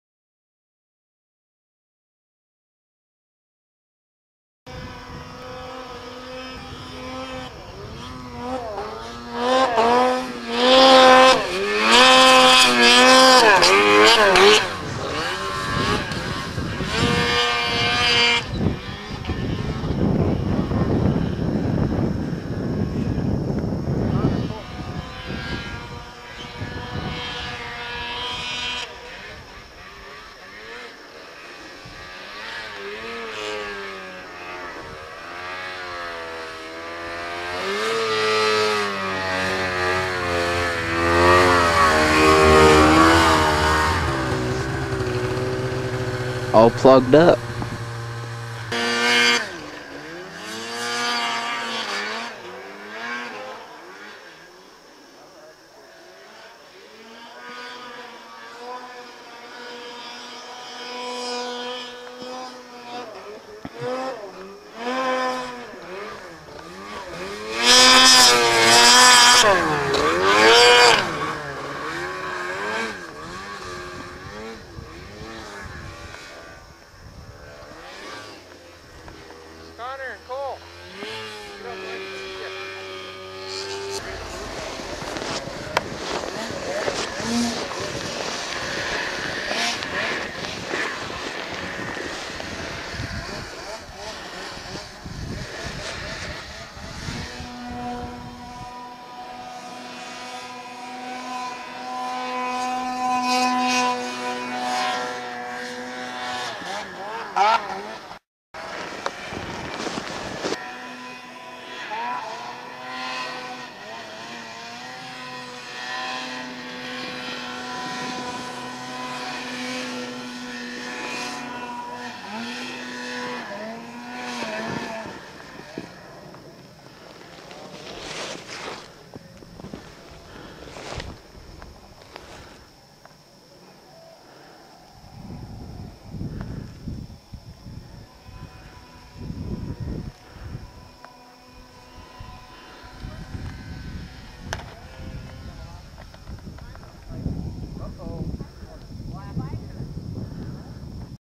Big Creek Jan 2009
Quick clip from Big Creek Meadows over new years 2009.  Snow was deep and light.  No editing done.  Straight from the camera.